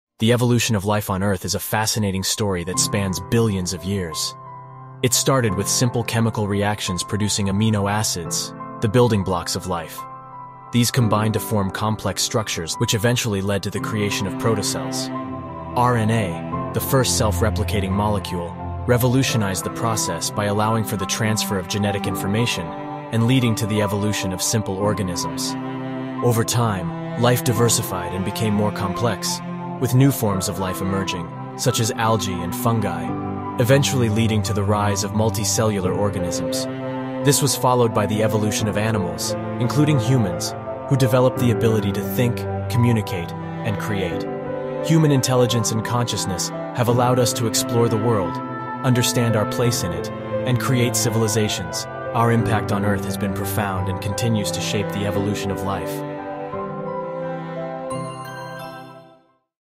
A Brief History Of Life
We explore the fascinating history of life on Earth. From the earliest single-celled organisms to the complex species that roam our planet today, we'll take a look at the key events and milestones that have shaped the evolution of life as we know it.